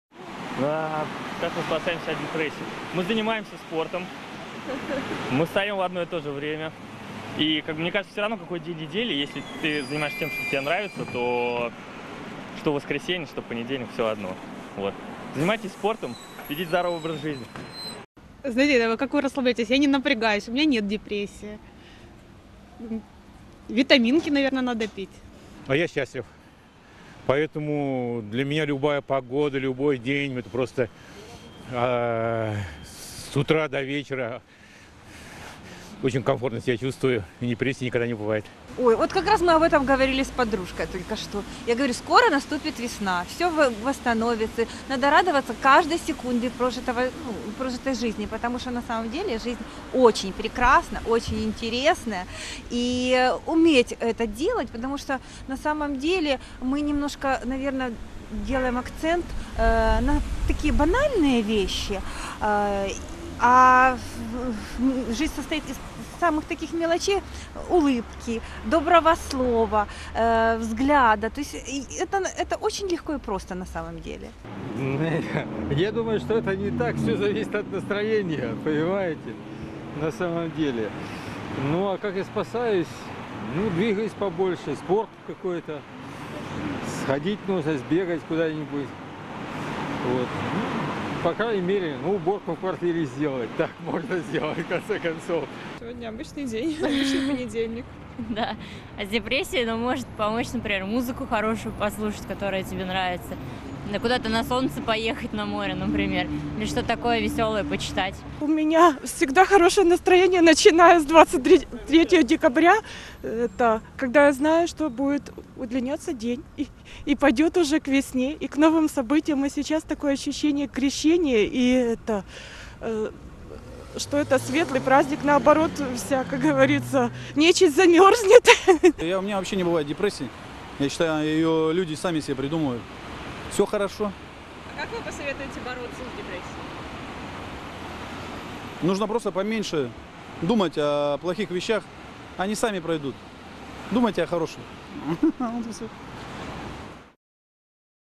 Как москвичи и киевляне борются с депрессией?
Третий понедельник января многие называют Blue Monday - "грустный понедельник". Это название закрепилось с легкой руки психолога из Кардиффа Клиффа Арналла, который высчитал, что сочетание плохой погоды, падения мотивации после рождественских каникул и накопившихся долгов делает этот день самым депрессивным в году.